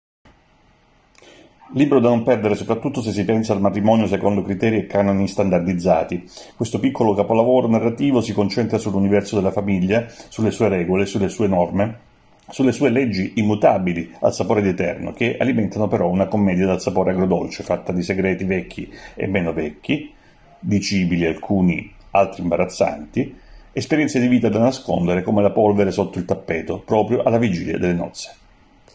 Matrimonio in cinque atti Condividi di Leah Hager Cohen (Sur)
Libro da non perdere soprattutto se si pensa al matrimonio secondo criteri e canoni standardizzati. Questo piccolo capolavoro narrativo si concentra sull’universo della famiglia, sulle sue regole, sulle sue norme, sulle sue leggi immutabili al sapore di eterno, che alimentano però una commedia dal sapore agrodolce fatta di segreti vecchi e meno vecchi, dicibili alcuni , altri imbarazzanti, esperienze di vita da nascondere come la polvere sotto il tappeto, proprio alla vigilia delle nozze.